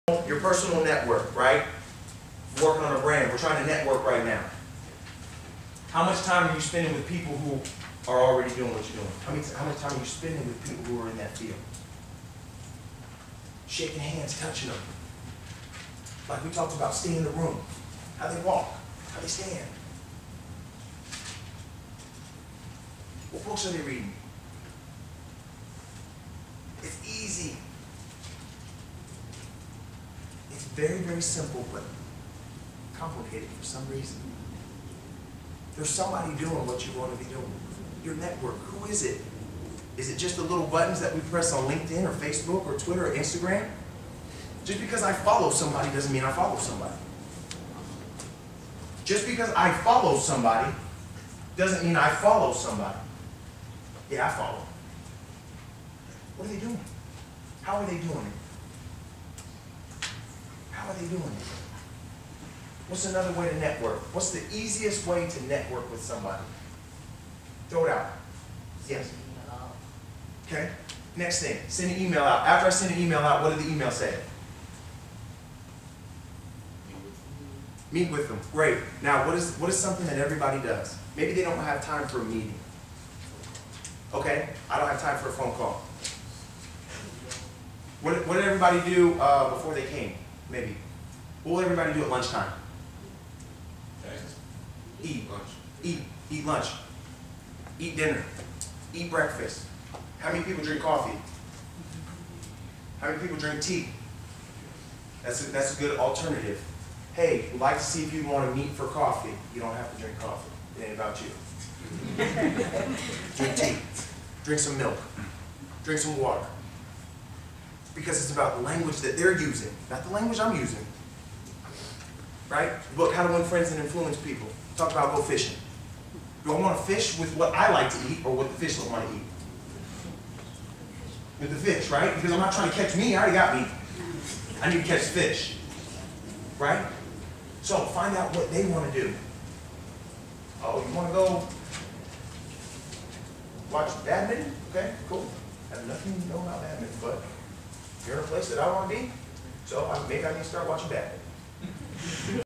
Thomas R. Williams: YearUP-AZ: Tips for networking
Thomas R. Williams speaking at YearUp Arizona: "Tips for networking"